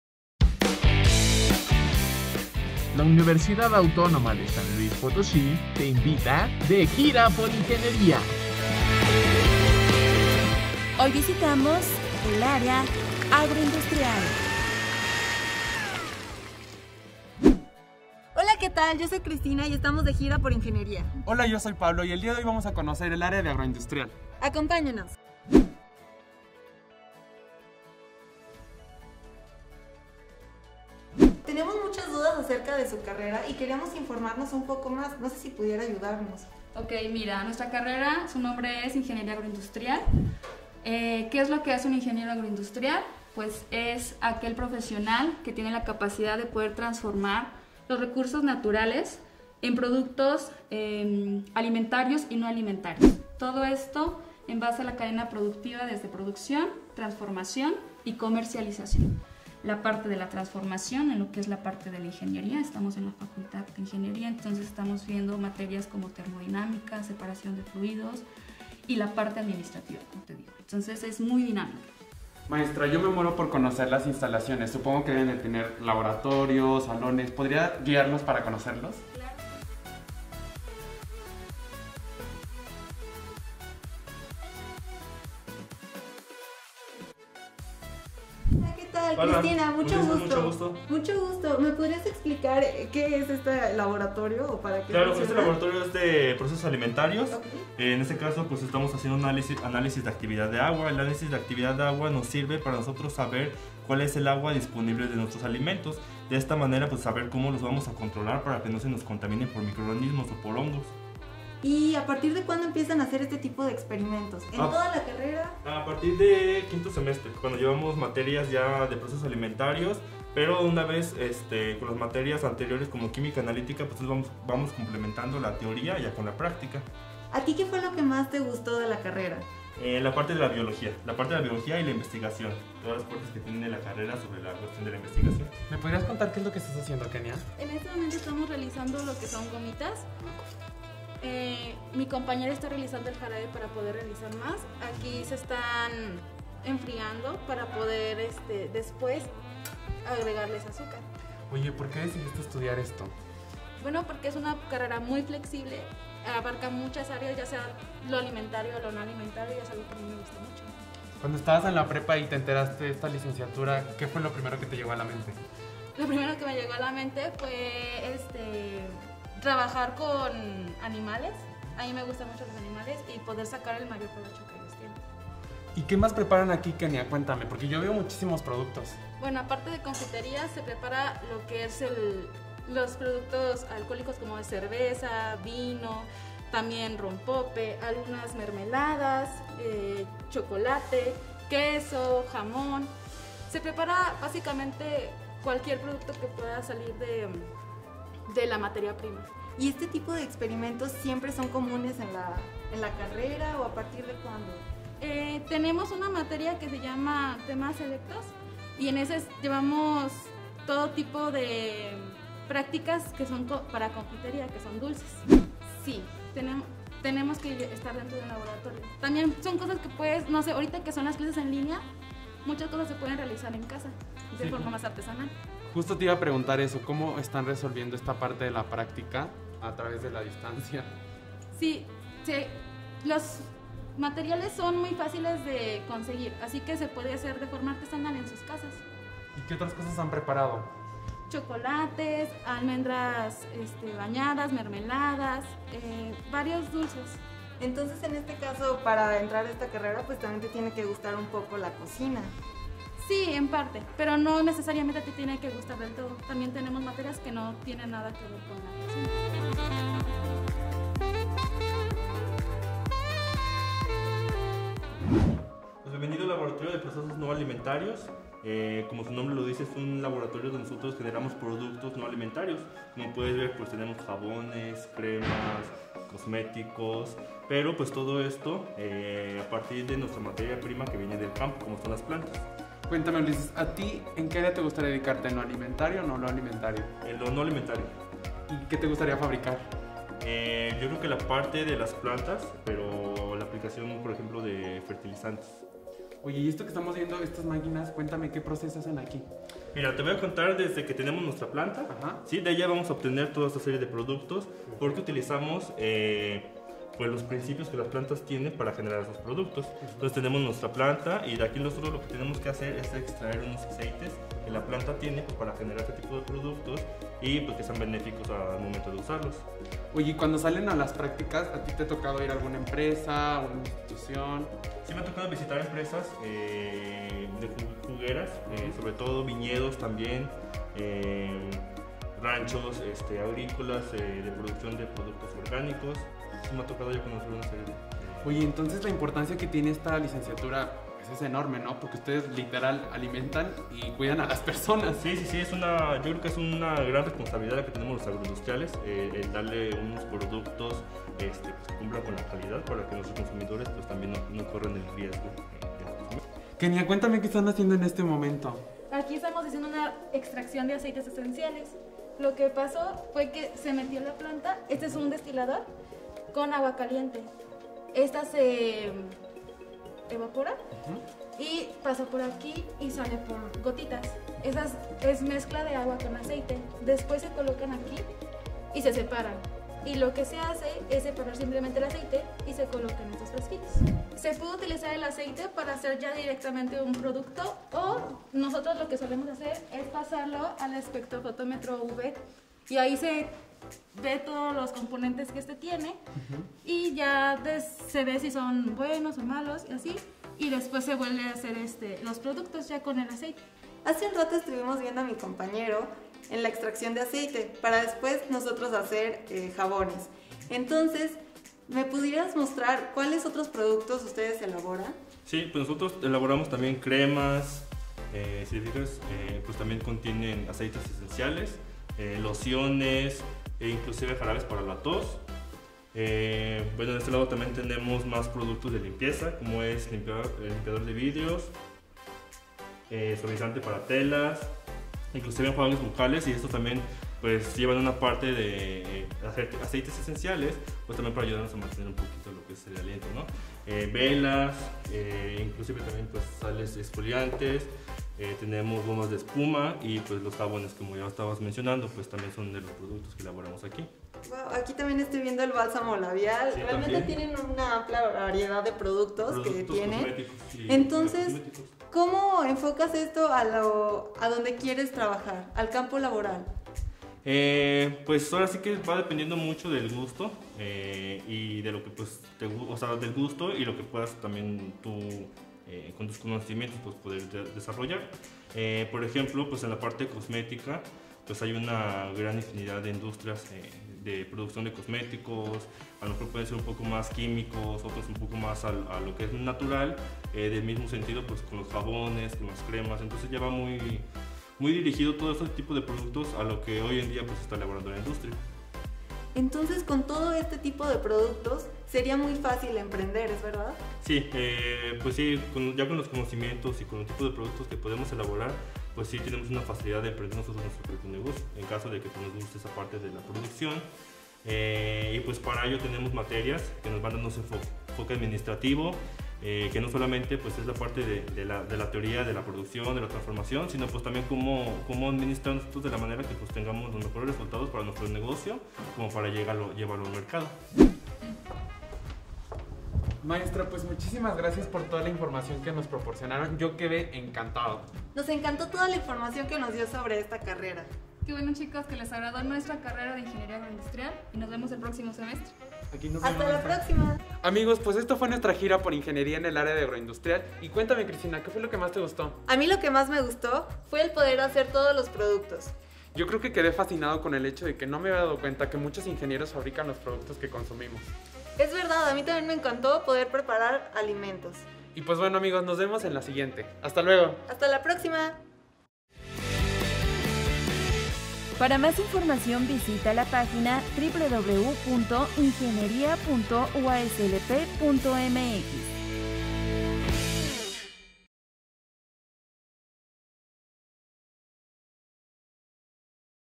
De Gira Por Ingeniería _ Área Agroindustrial
Acompáñanos deGira por Ingeniería para conocer el Área Agroindustrial de la Facultad de Ingeniería de la Universidad Autónoma de San Luis Potosí